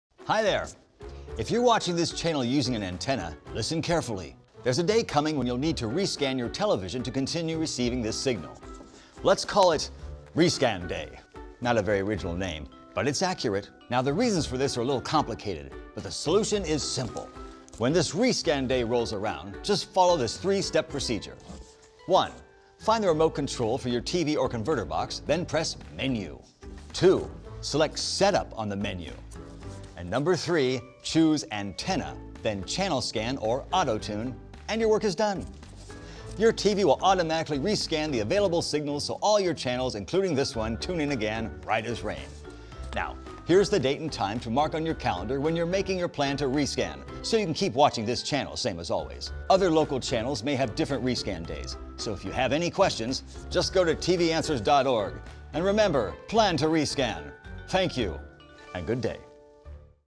Rescan Your Channels on March 10, 2020 | WGCU-TV
If you are one of the more than 72 million Americans who rely on an antenna to watch free, over-the-air television, you'll need to rescan your television set when your local stations move to new TV frequencies on March 10, 2020. Rescanning is when your TV finds all of the available channels in your area.

No new equipment is needed. If your TV is connected to cable or satellite service, you don’t need to do anything.

Plan to Rescan!
Select "scan" or "autotune" from your TV or converter box control menu to start the scanning process. You can usually find instructions by pressing the "set-up" or "menu" buttons on your television or digital antenna remote control. If you have difficulty, consult the owner's manual for more detailed instructions on how to run a channel scan, or contact customer service for your TV or converter box manufacturer.

After you rescan your TV set, the channel number for your local TV station will remain the same. If you haven’t rescanned your TV recently, you may discover TV channels that you didn’t know you were missing.

How do I know when to rescan?
TV stations must move frequencies at different times starting in the fall of 2018 through at least the spring of 2020, so if you watch TV for free using an antenna you may need to rescan more than once.

For additional help, please contact the Federal Communications Commission’s hotline at 1-888-CALLFCC and press "6" to speak to a help desk representative. The call center is staffed from 8 a.m. to 1 a.m. Eastern time, seven days a week.